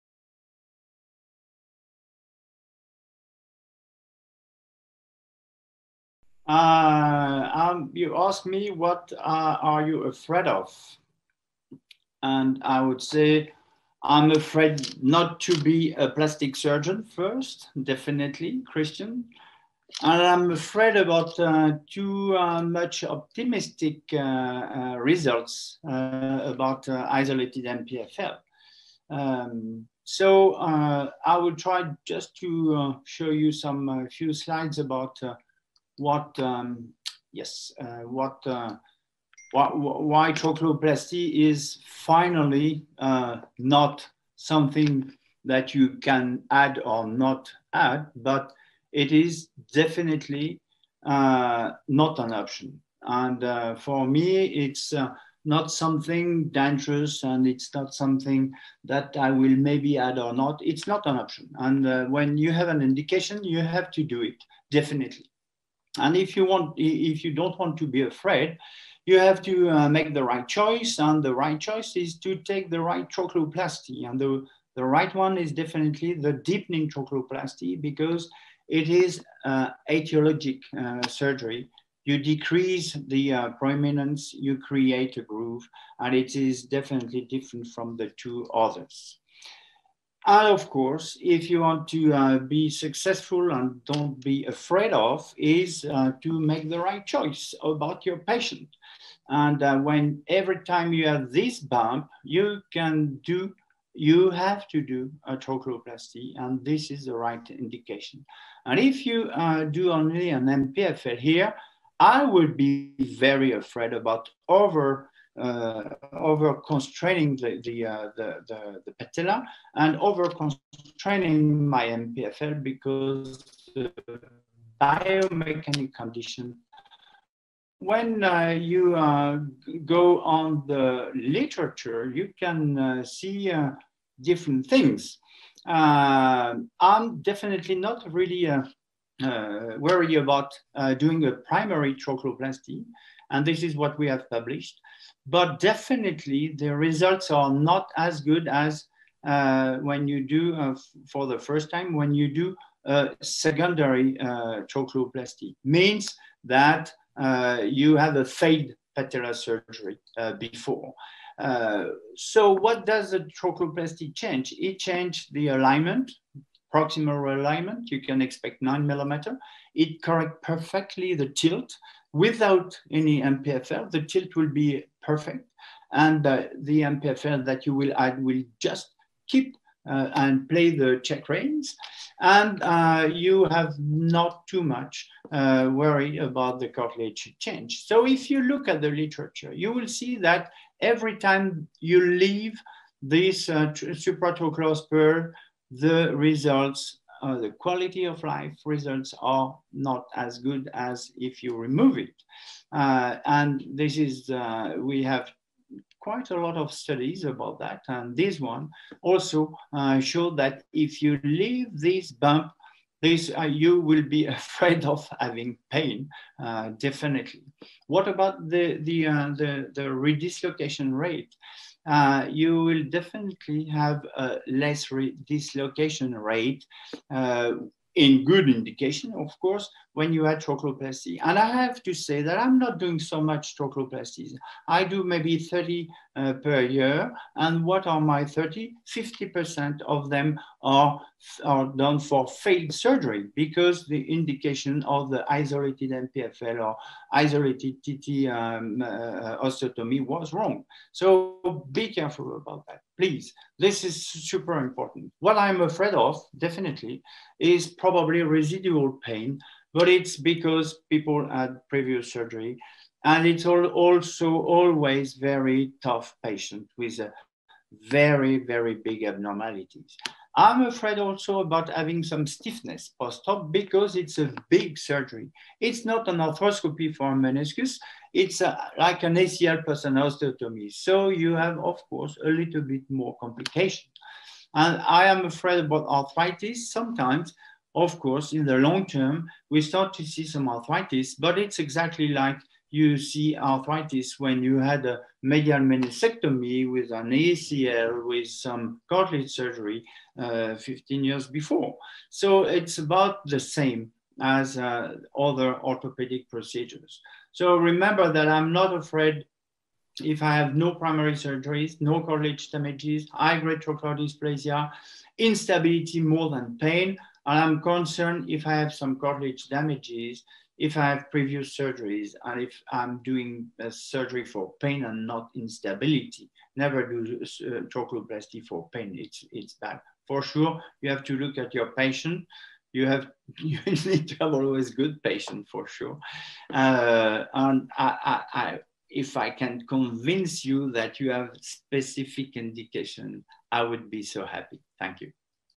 Pro: Add A Trochleoplasty: What Are You Afraid Of? - David DeJour, MD
This video is from the 2020 Orthopaedic Summit Evolving Techniques.

If you'd like to learn more about Patellar Instability, click here to view the topic on Orthobullets